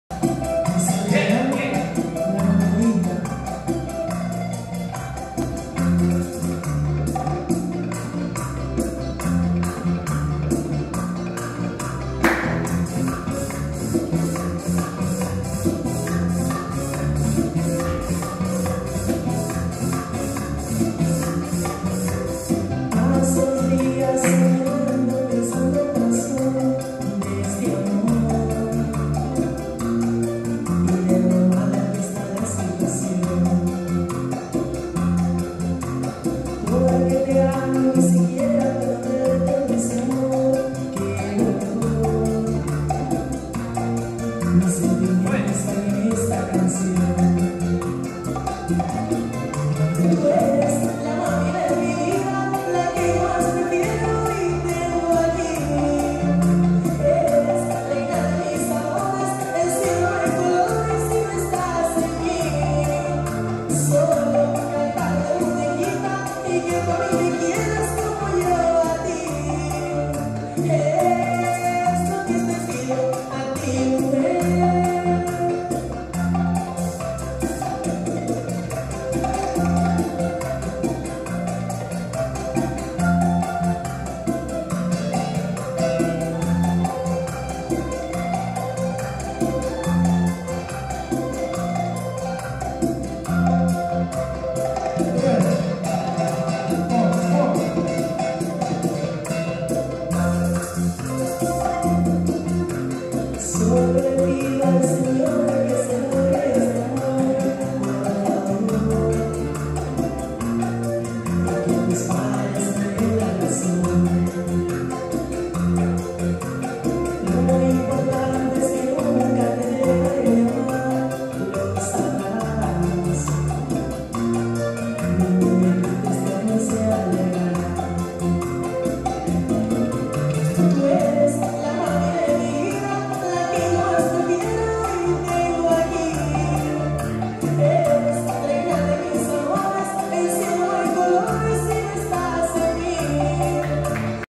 Saturday's Bachata class by Ashe's Bachateros
Shines sequence and partnership work, traditional bachata